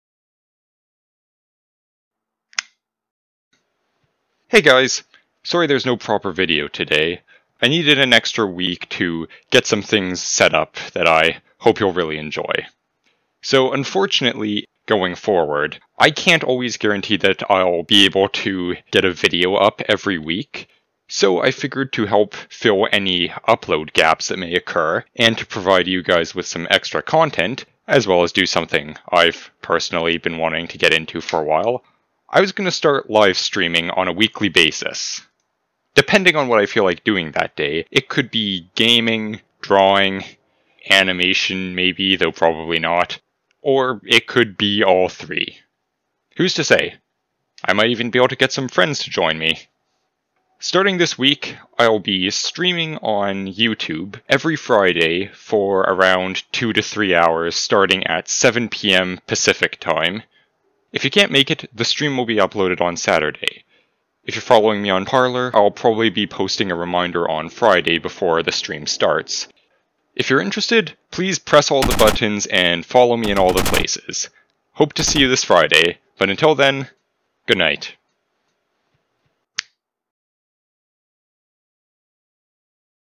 Taking a Break (But Not Really)
New plans for the channel after a slightly hectic past few weeks. Hope you'll join me in this new direction. Don't worry, I'll still be making videos, they'll just be less frequent in order to give me time to not work myself to death.